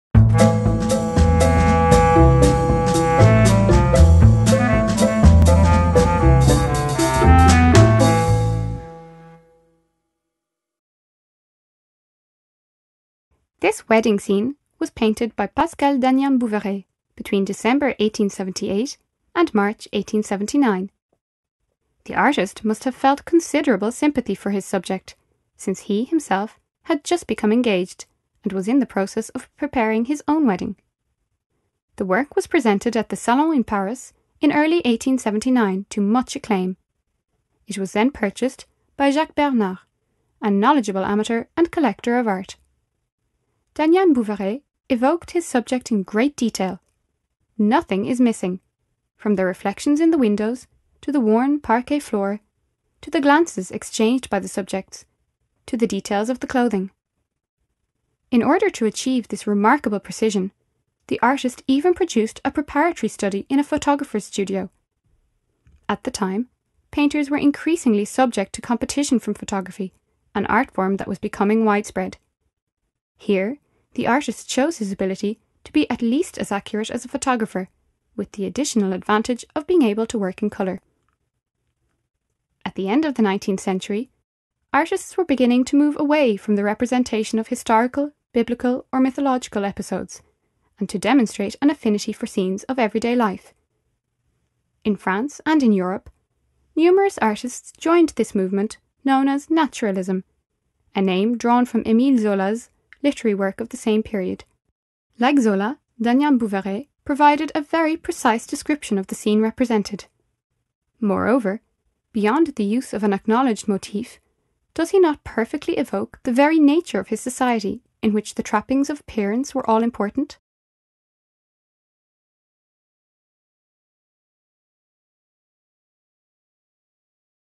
Dagnan A Wedding party, in gigapixel
Explore a 19th century wedding ceremony by Pascal Dagnan-Bouveret in ultra-high resolution !